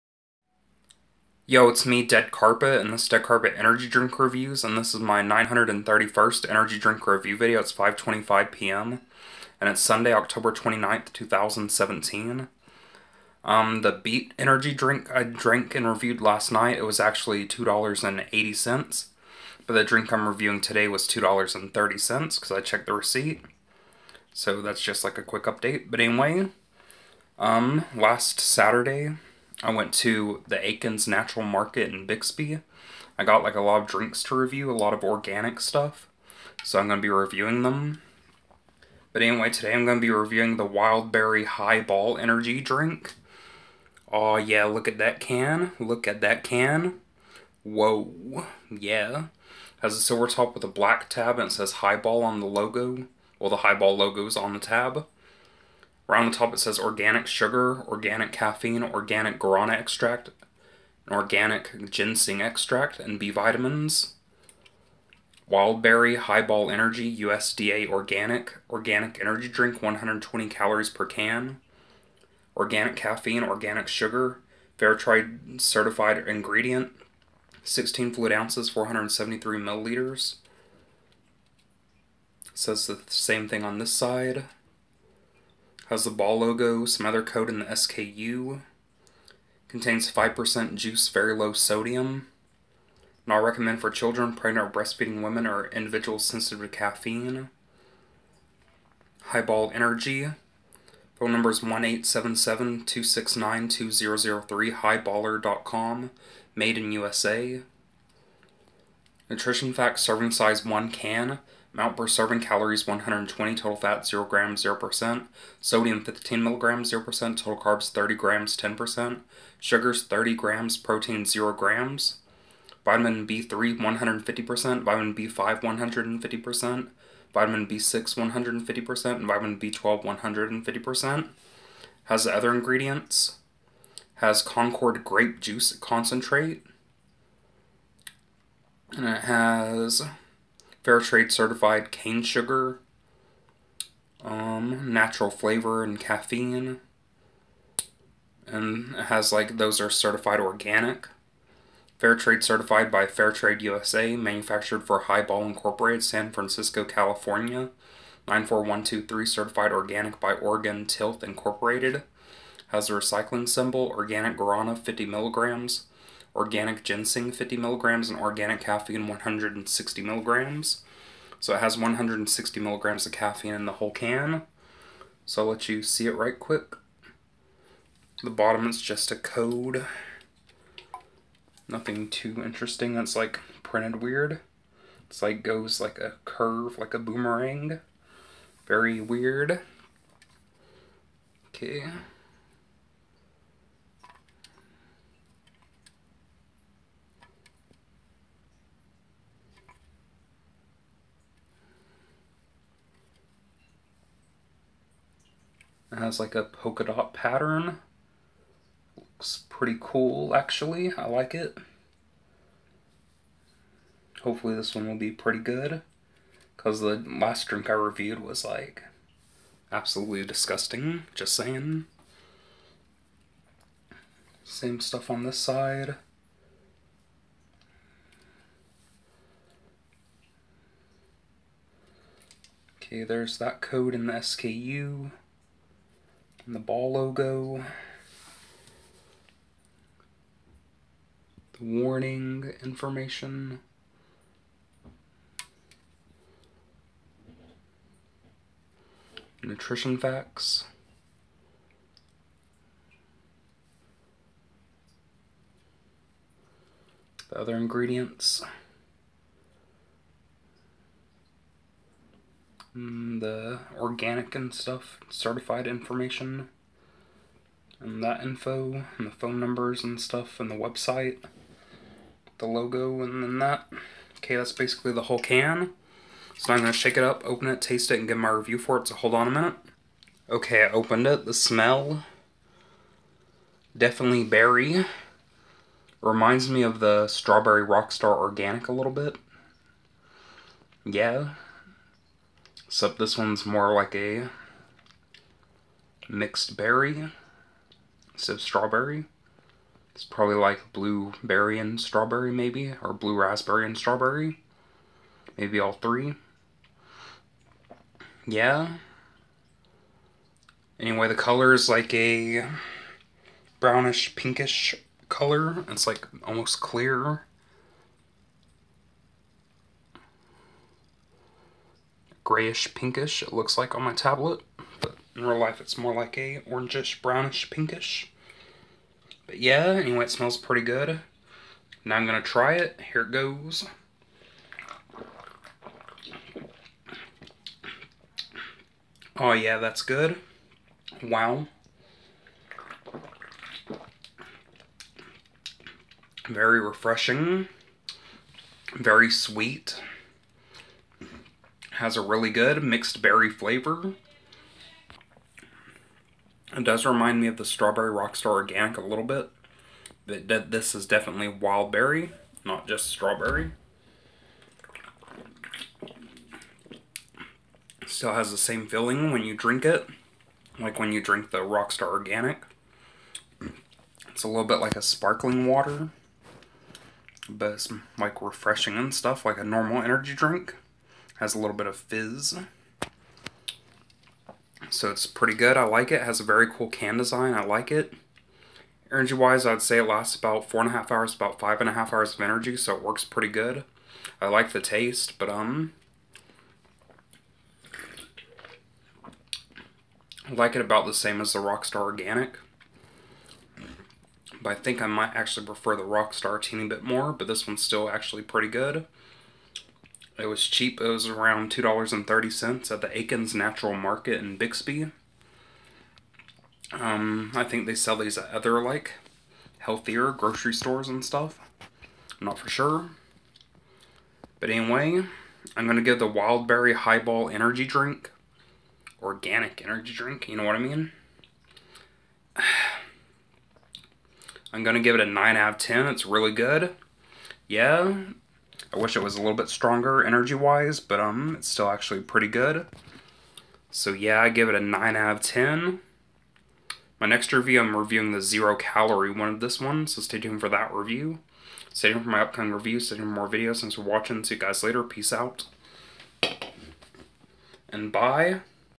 Deadcarpet Energy Drink Reviews - Wild Berry Hiball Organic Energy Drink
I review the Wild Berry Hiball Organic Energy Drink. This is my 931st energy drink review video.